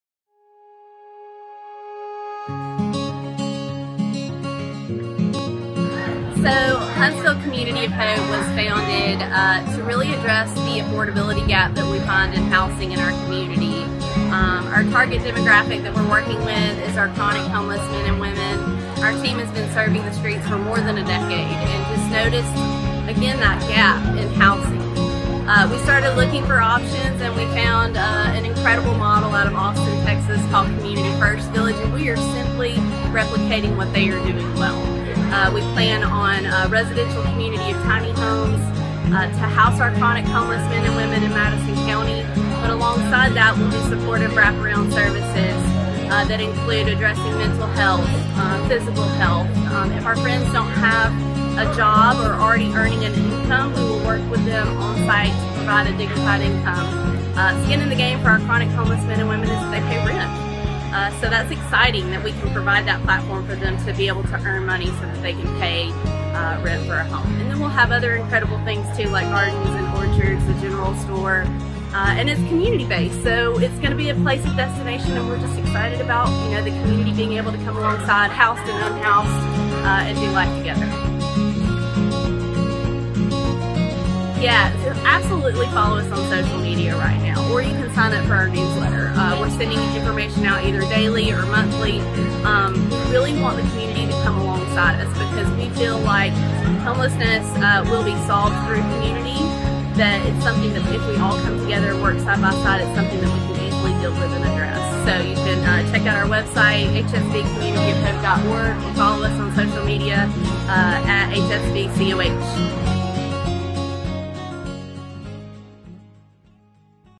Grant Winner Spotlight: Huntsville Community of Hope
Huntsville Community of Hope was founded and established in 2019 to build a residential community for the chronically homeless and empower the surrounding community into a lifestyle of love and service.

The $10,000 grant will continue the goals of the COH to build a self-supportive, residential community of tiny homes designed to not only lift our homeless friends off the streets but to keep them off the streets. Mental Health, Physical Health, Education, Job-Development, Recovery Programs, Farming, Arts and Culture, Basic Needs, Economic Development, Recreation, Transportation, and Well Being Programs will all be available upon completion of the community.
Member FDIC. Equal Housing Lender